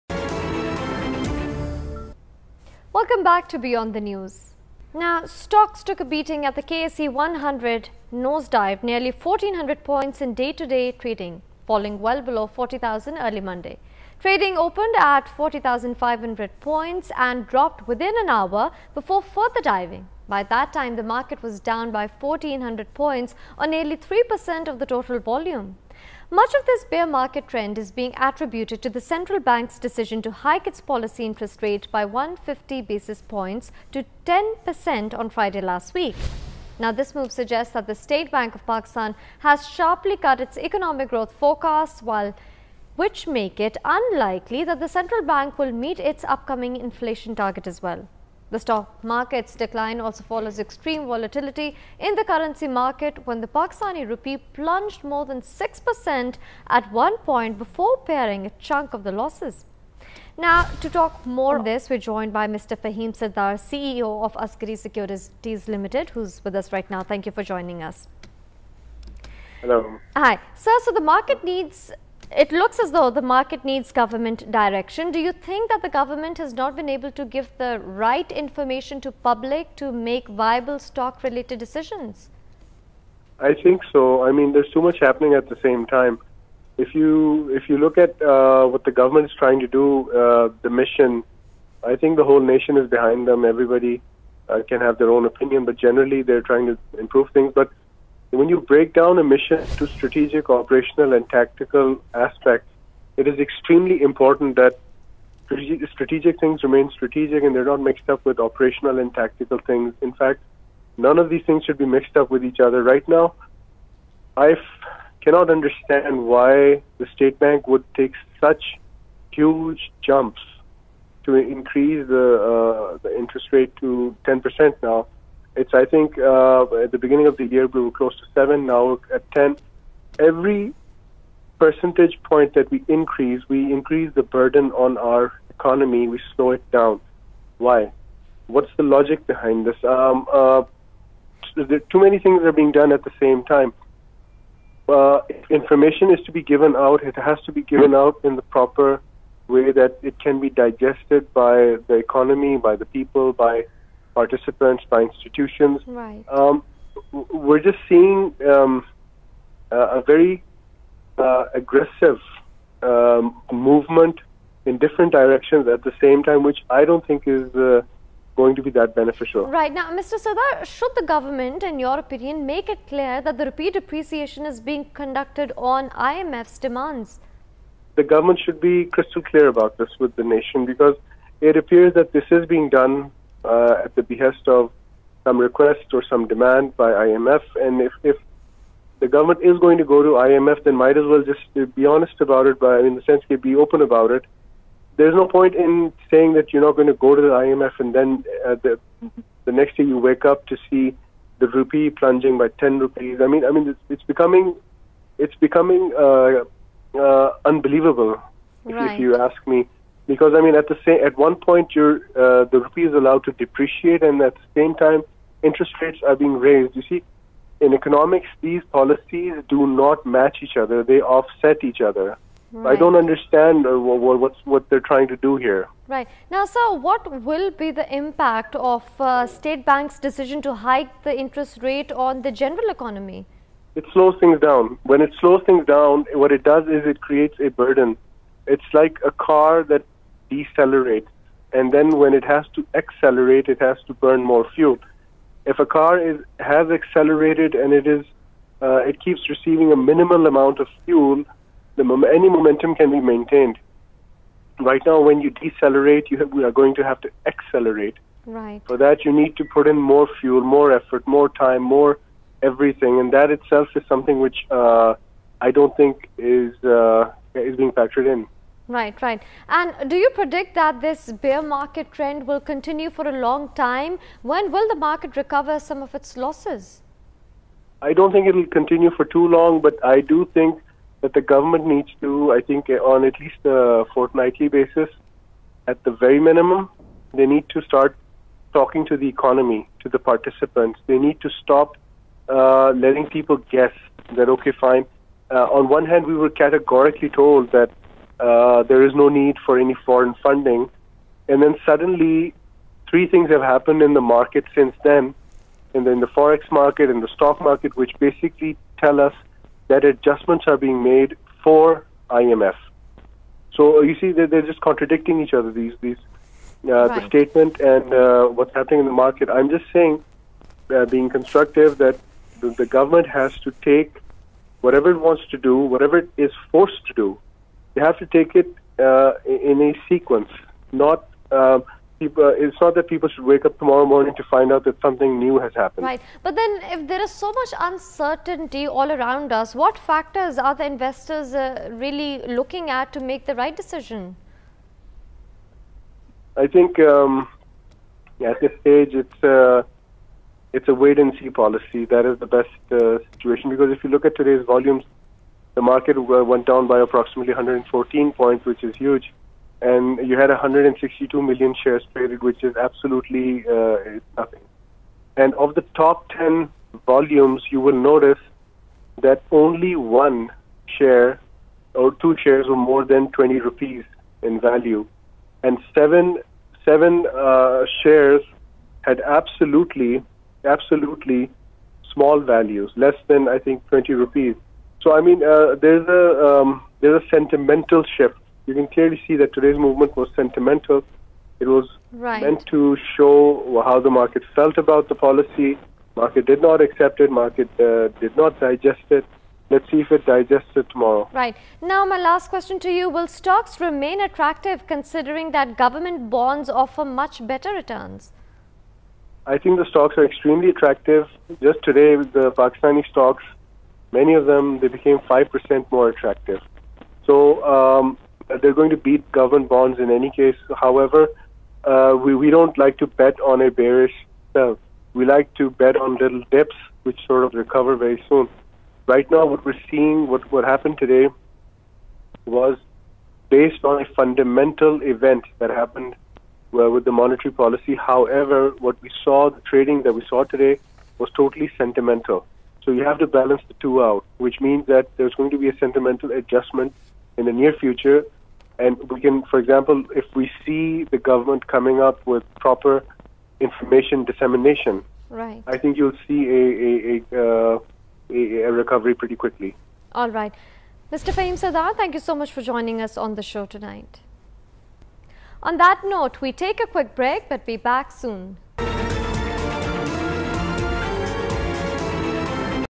Pakistan Stock Market Latest - Market Fall, IMF & Interest Rate Hike | Faheem Sardar 20181203
The Latest on the Pakistan Stock Market, with the market taking a sharp fall, following the interest rate hike and vacuum of government news.